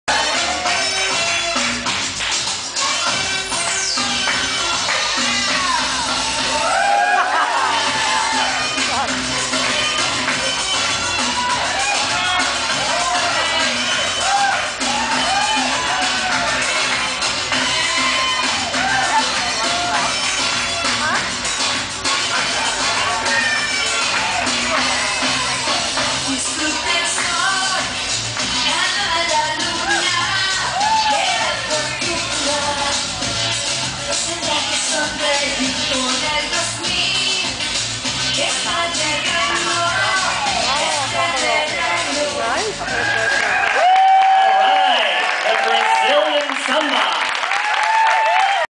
International Style Samba at California Dreaming 2008
Same-sex dancers compete at California Dreaming 2008 in Oakland, CA.